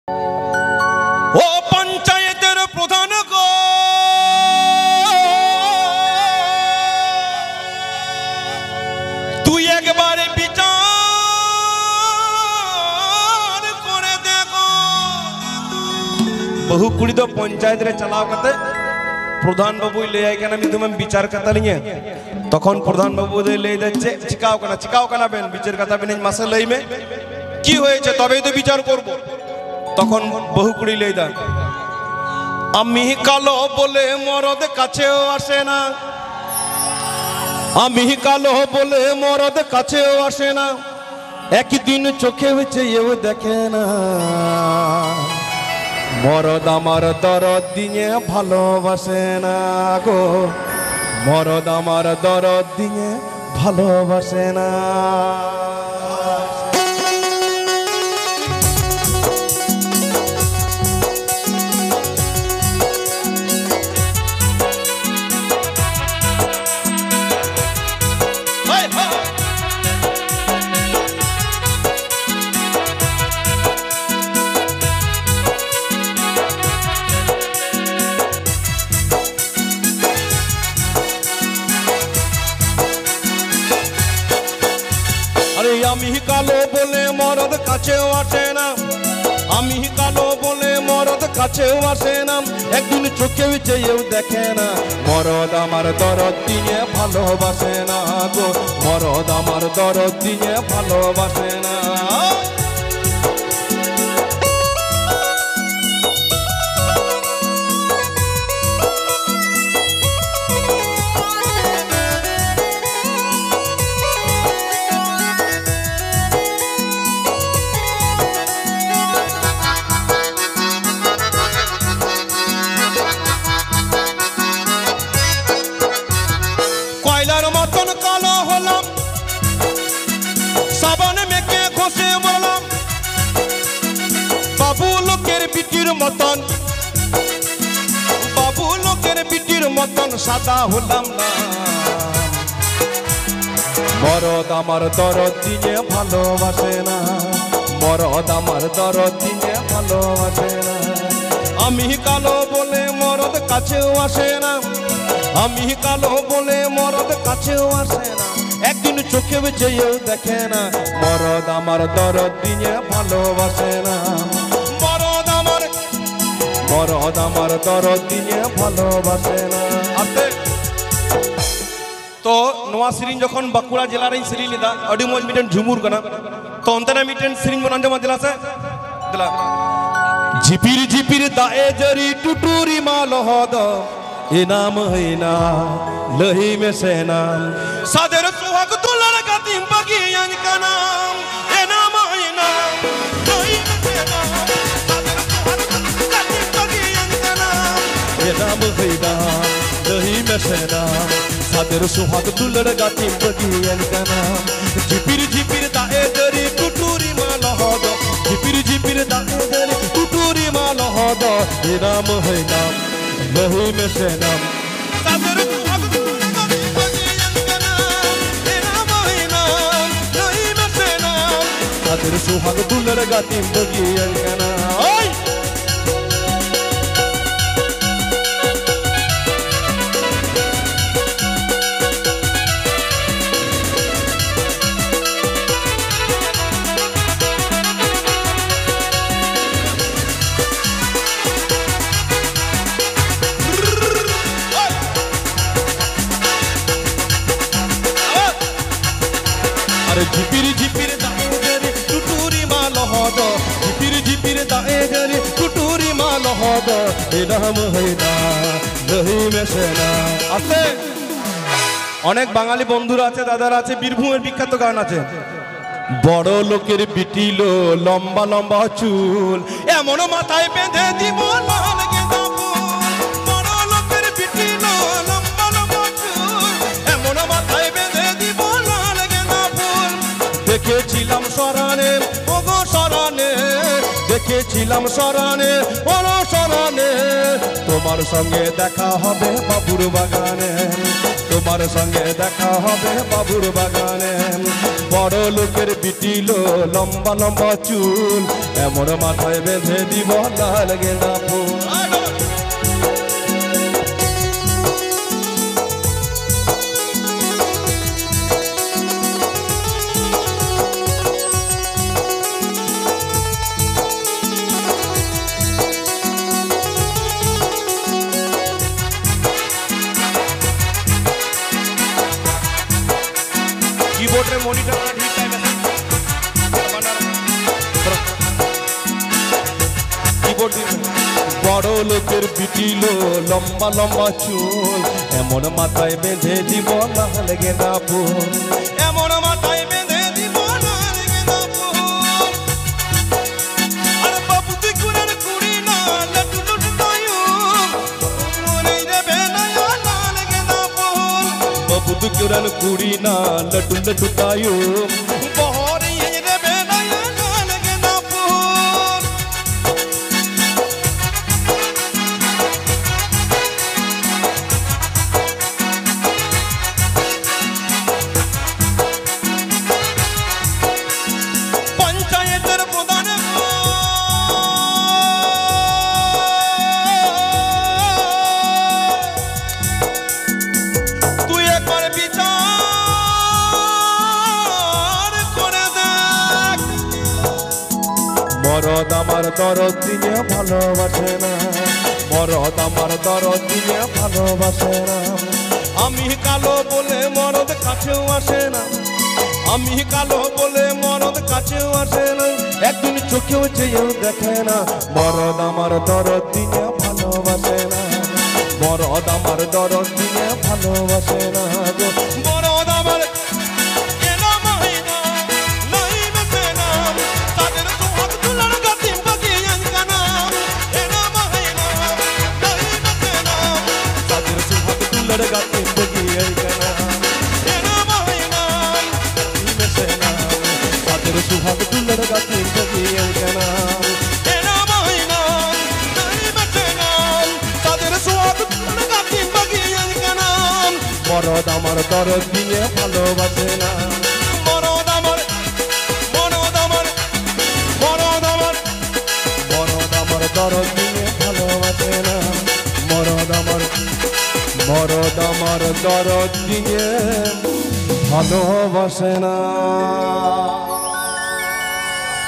Rathin Kisku Stage Program Video 2024//Nonstop Dong Orchestra Music Video
All Category Santali Videos Are Here | Akhara Tala Rushika

Singer: Rathin Kisku
Pad:Diwan
Organ: Parmeshwar
Tumdak:Kalidas

👇ROSE SOUND M.D BAZAR BIRBHUM W.B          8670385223